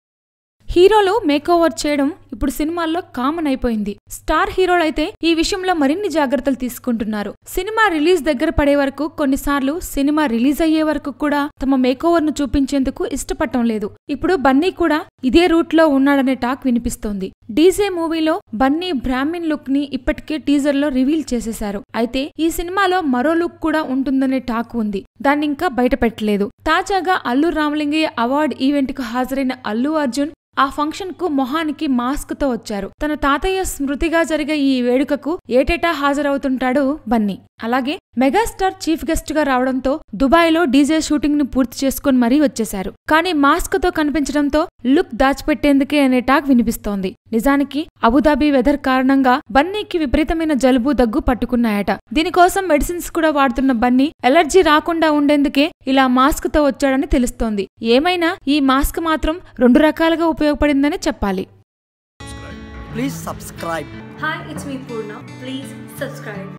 Allu Arjun Spotted At Allu Ramalingaiah Memorial Award Presentation Event With Mask | Telugu Cinema
Watch Allu Arjun Spotted At Allu Ramalingaiah Memorial Award Presentation Event With Mask On Telugu Cinema